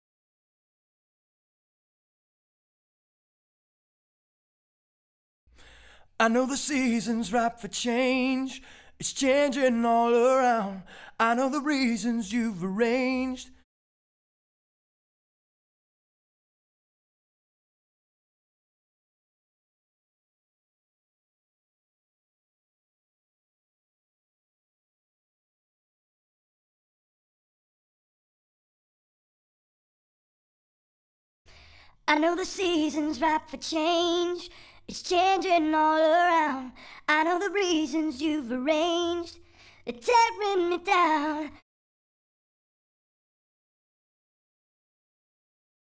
How to Transpose An Audio Channel | n-Track Studio
A short tutorial about how to transpose an audio track in n-Track Studio.